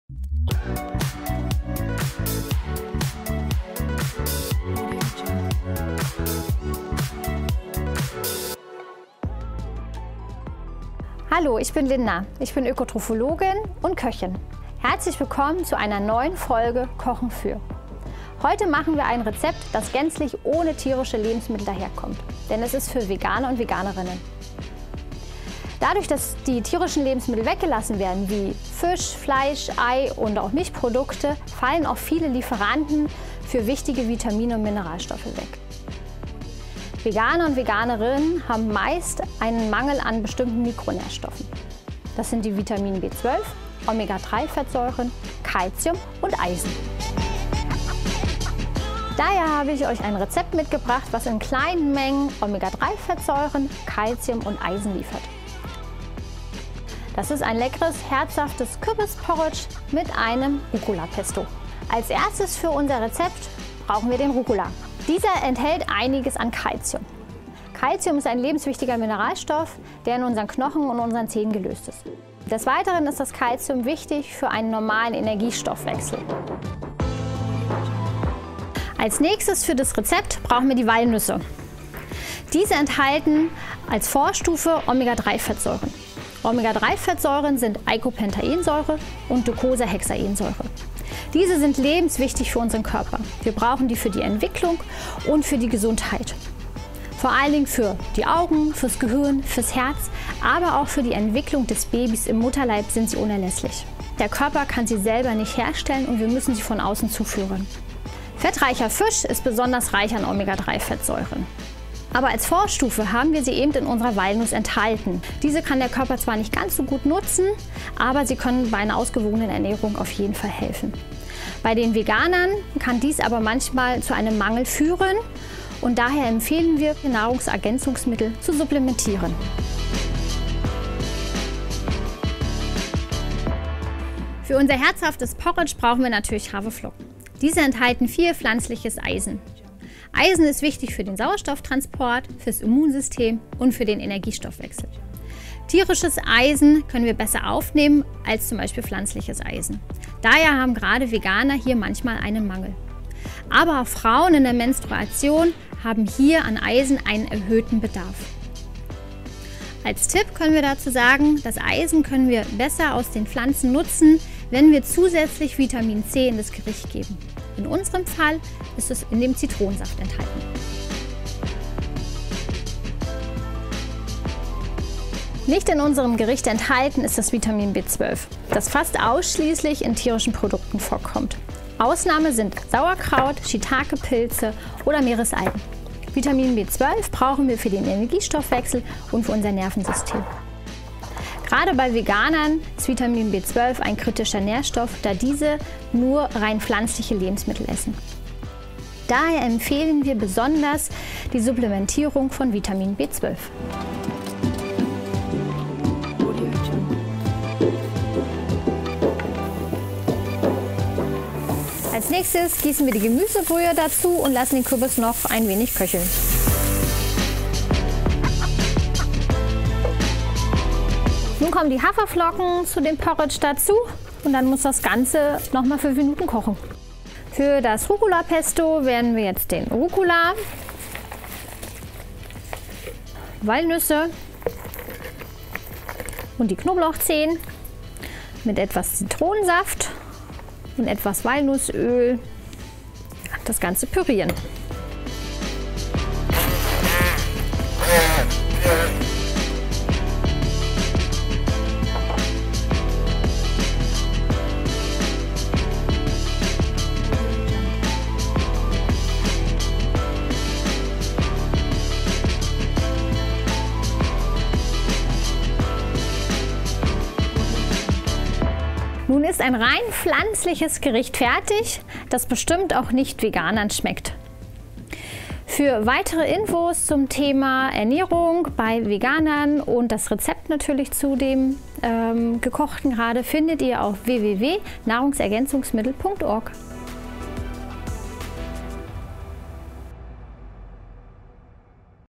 Kochen für Veganer:innen
Wer sich vegan ernährt, muss auf einige Mikronährstoffe besonders achten, denn wenn keine tierischen Lebensmittel wie Milch, Eier, Fleisch oder Fisch auf dem täglichen Speiseplan stehen, fallen auch viele gute Lieferanten für Vitamine und Mineralstoffe weg. Deshalb ist es für Veganerinnen und Veganer besonders wichtig, dass sie sich aus der großen Vielfalt der pflanzlichen Lebensmittel bedienen und auf eine ausreichende Nährstoffzufuhr achten. Der Augenmerk sollte dabei insbesondere auf folgende Mikronährstoffe liegen: Vitamin B12, Omega-3-Fettsäuren, Calcium und Eisen. Oecotrophologin und Köchin Linda Otto hat ein Gericht gezaubert, das genau diese kritischen Nährstoffe beinhaltet und so dazu beitragen kann, die notwendige Zufuhr sicherzustellen: Ein herzhaftes Kürbis-Porridge mit Rucola-Pesto.

Dir hat das Video gefallen und du möchtest mehr von uns sehen?